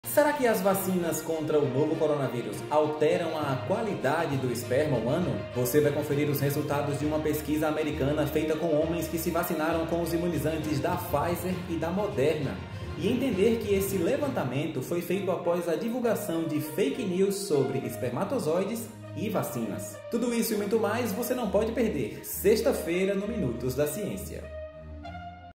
Chamada Minutos da Ciência (30/07/2021)
Você vai descobrir conosco se as vacinas contra a Covid-19 alteram ou não o esperma humano.